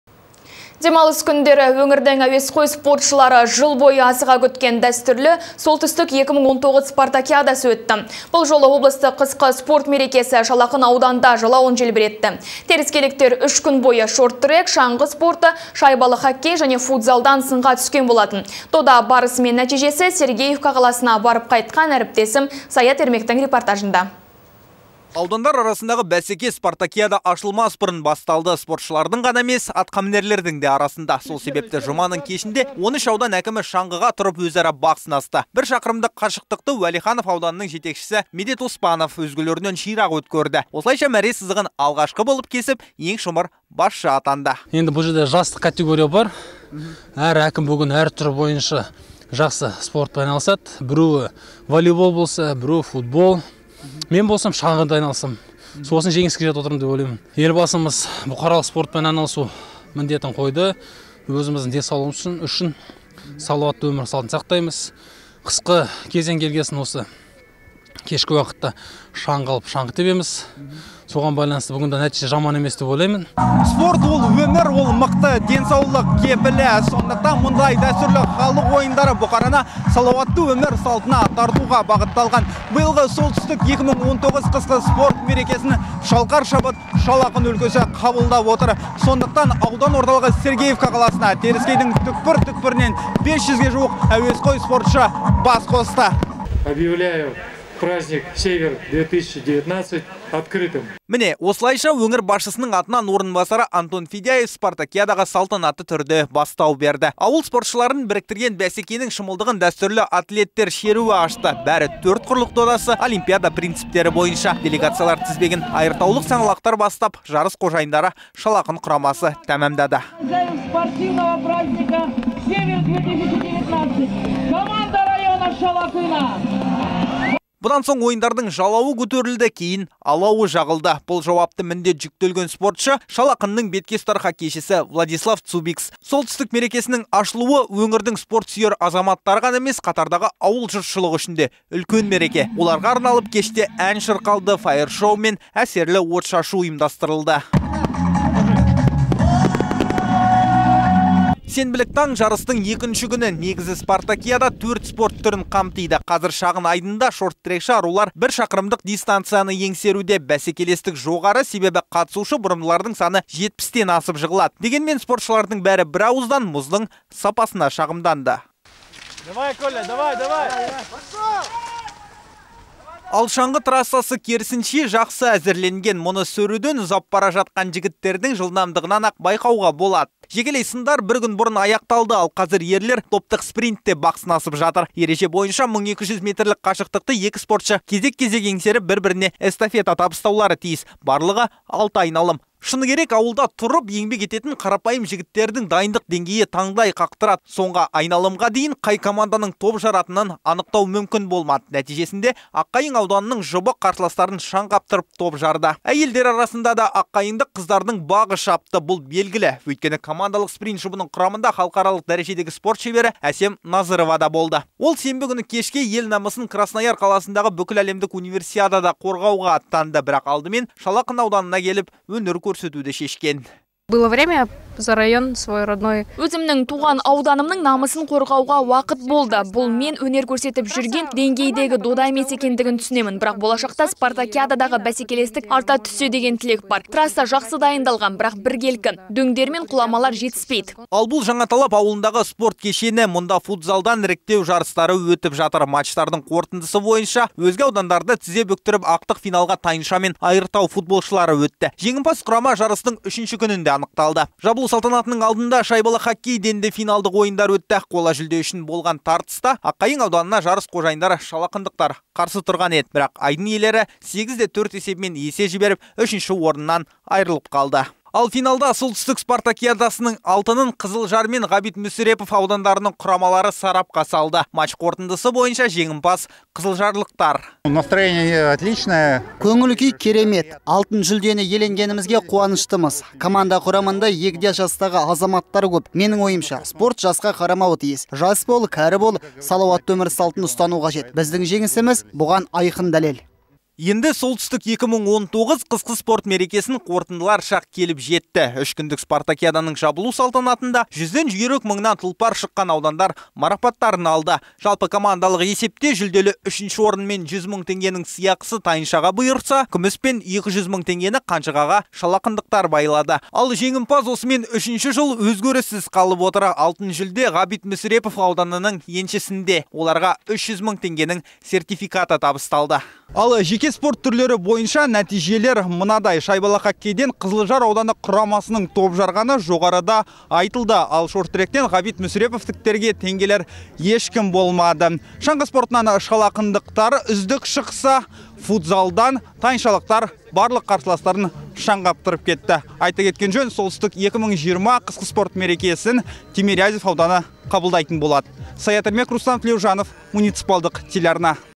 Шал ақын ауданы "Солтүстік-2019" қысқы спорт мерекесін қабылдады
Демалыс күндері / өңірдің әуесқой спортшылары жыл бойы асыға күткен / дәстүрлі "Солтүстік-2019" спартакиадасы өтті. Бұл жолы облыстық қысқы спорт мерекесі Шал ақын ауданында жалауын желбіретті. Теріскейліктер үш күн бойы шорт-трек, шаңғы спорты, шайбалы хоккей және футзалдан сынға түскен болатын. Дода барысы мен нәтижесі Сергеевка қаласына барып қайтқан әріптесім Саят Ермектің репортажында.